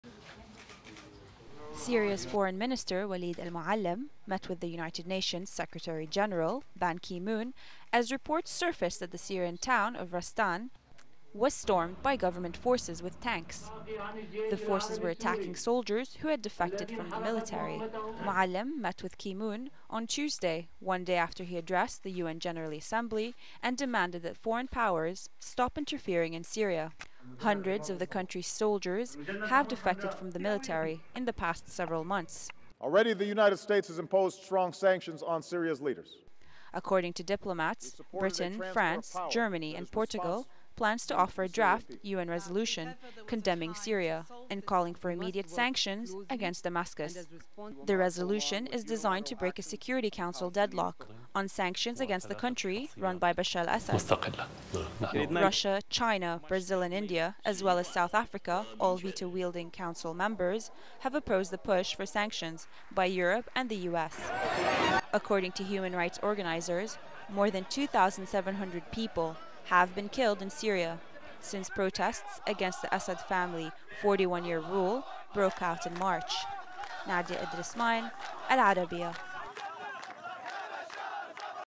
Syria's FM Meets with U.N. Chief
Syria's foreign minister, Walid al-Moualem, met with the United Nations secretary-general, Ban Ki-moon, as reports surfaced that the Syrian town of Rastan was stormed by government forces with tanks. The forces were attacking soldiers who had defected from the military.

Moualem met with Ki-moon on Tuesday, one day after he addressed the U.N. General Assembly and demanded that that foreign powers stop interfering in Syria.

Hundreds of the country's soldiers have defected from the military in the past several months.

According to diplomats, Britain, France, Germany and Portugal plans to offer a draft U.N. resolution condemning Syria and  calling for immediate sanctions against Damascus.

The resolution is designed to break a Security Council deadlock on sanctions against the country run by Bashar al-Assad.

Russia, China, Brazil and India, as well as South Africa, all veto-wielding council members, have opposed the push for sanctions by Europe and the U.S.

According to human-rights organizers, more than 2,700 people have been killed in Syria since protests against the Assad family's 41-year rule broke out in March.